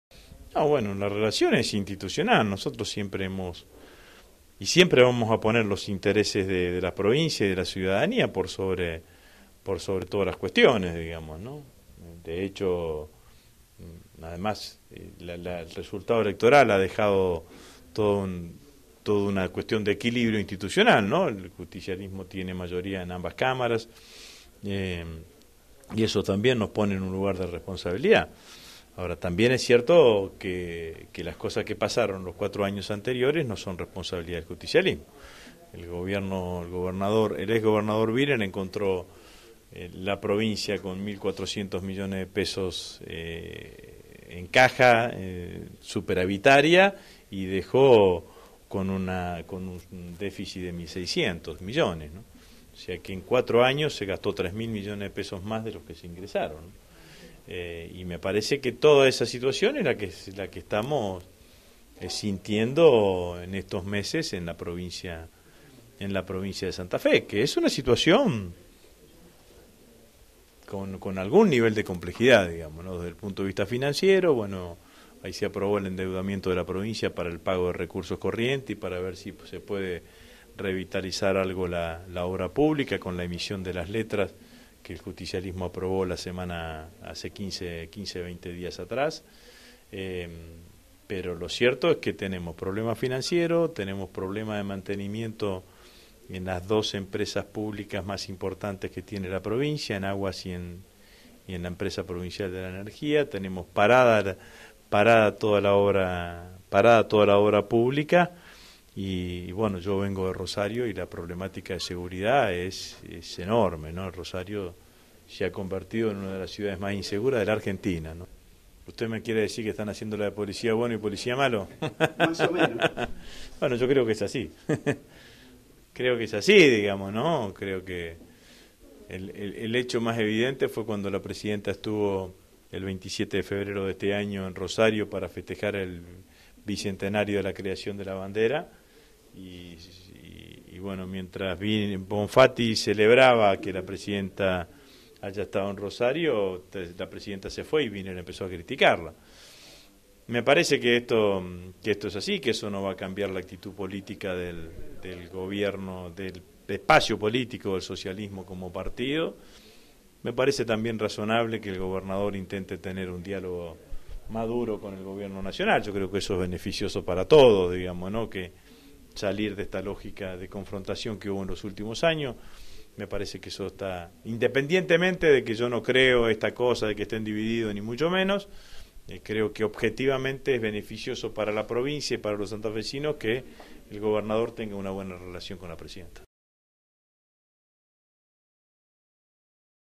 Agustín Rossi en San Vicente.
El presidente del bloque de diputados nacionales del Frente para la Victoria, Agustin Rossi, participó de la entrega de una ambulancia en San Vicente.
En este fragmento de la entrevista con Informe 2, hace referencia a la relación del gobierno provincial con la nación.